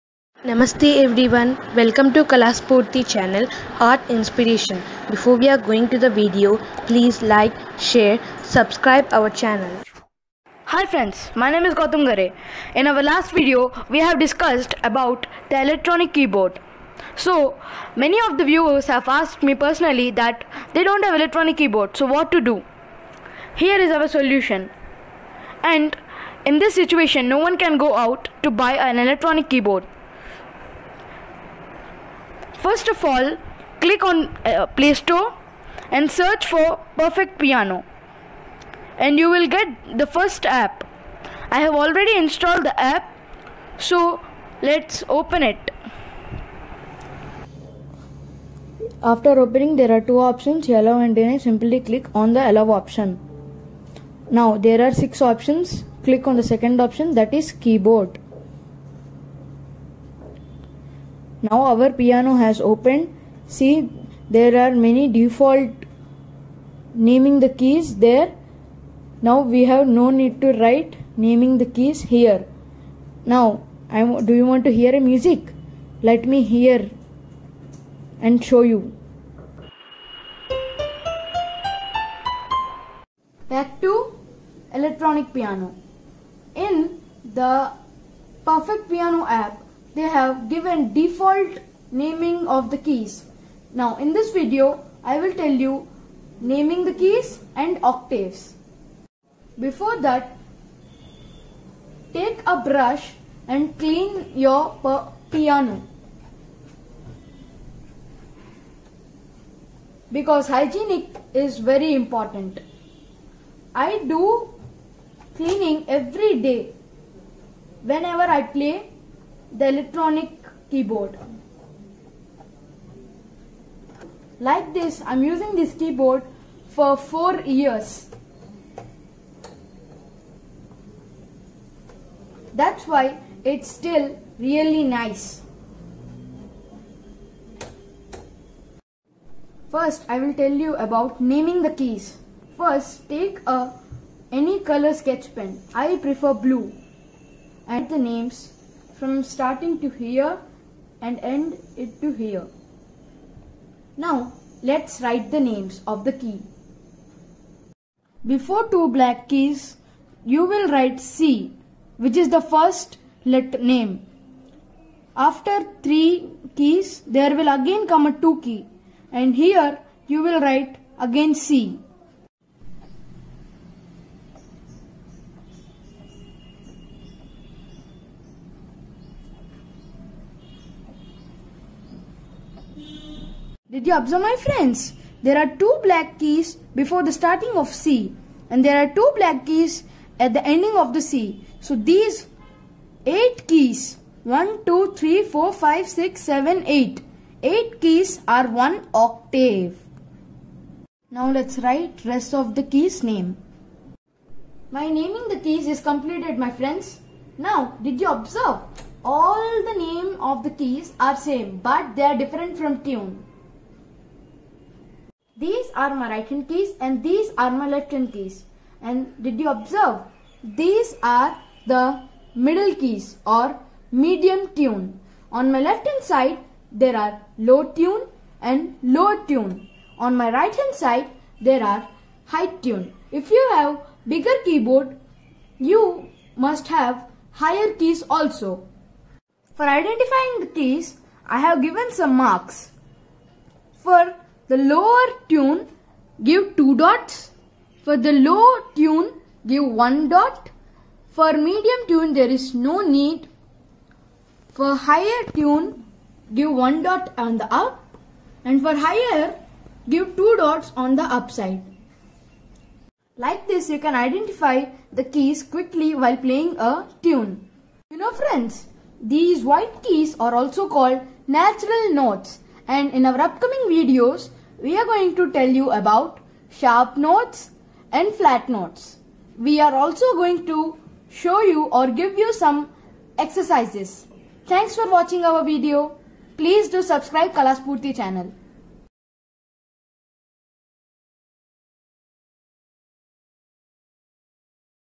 Basics for keyboard learning to Beginners with keyboard (OR) using Mobile App (Part 2)
Hello friends,
Here is the video on Keyboard very basic notes.
The best online course for beginners to learn to play the piano or electronic keyboard.Easily u can learn from mobile app also.